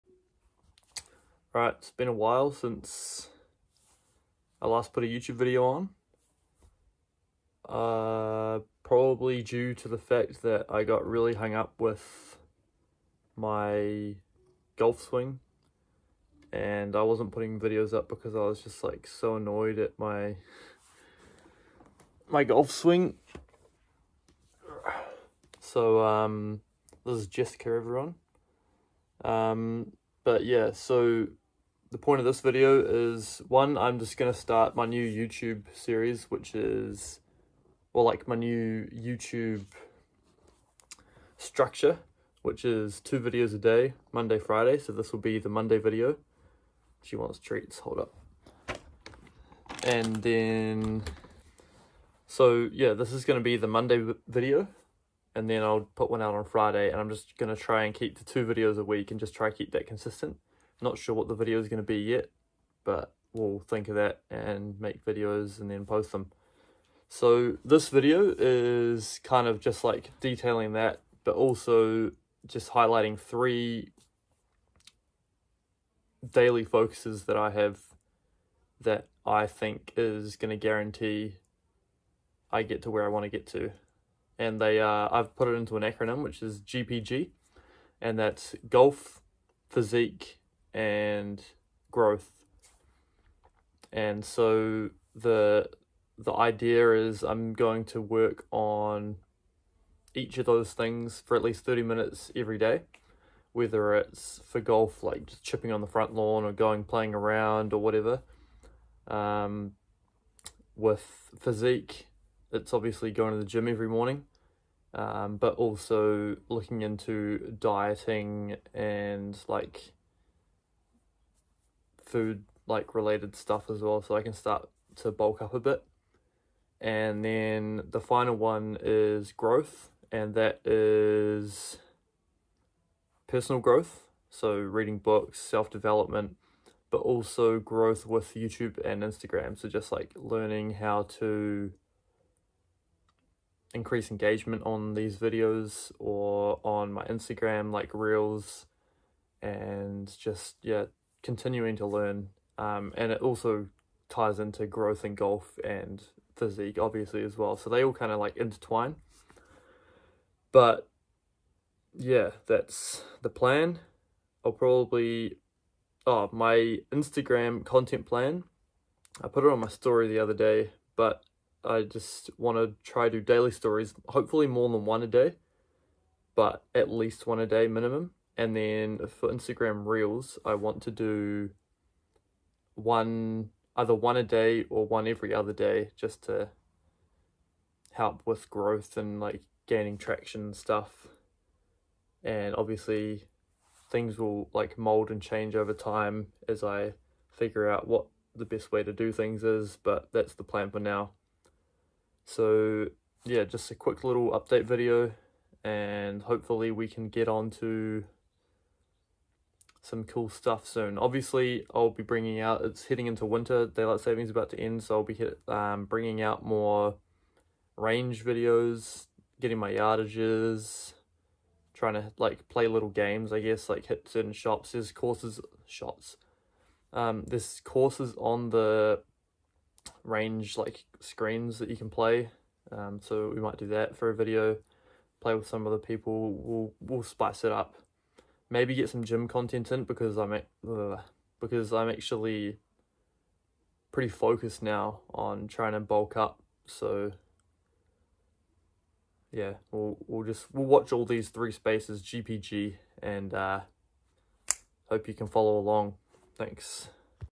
GPG: Daily Focuses + Content update
Just a quick video outlining what’s to come and how I’m planning on getting to where I want to be in 3 years time.